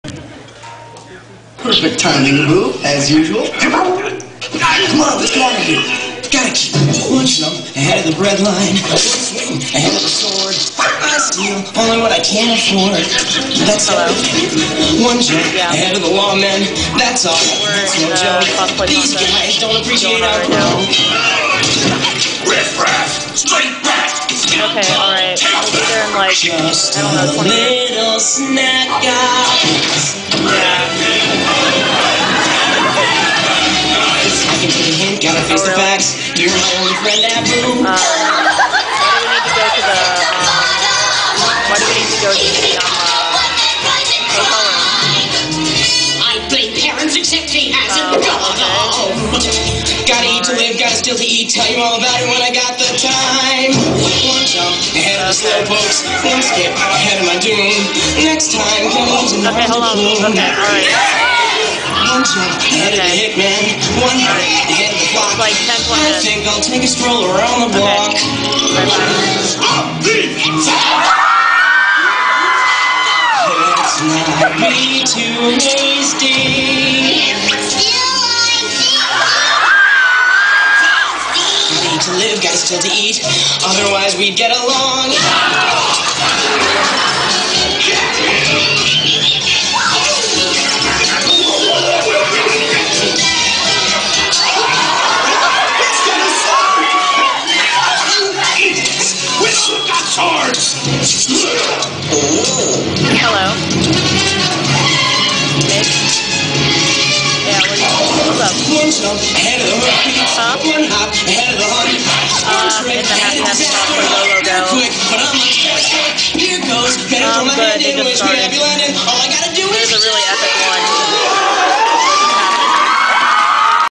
Kingdom Hearts: 'Musical'
This was filmed at Happy Happy Cosplay Go Go Go! at AFO.
Sorry for the crappeh-esque video! I tried my best.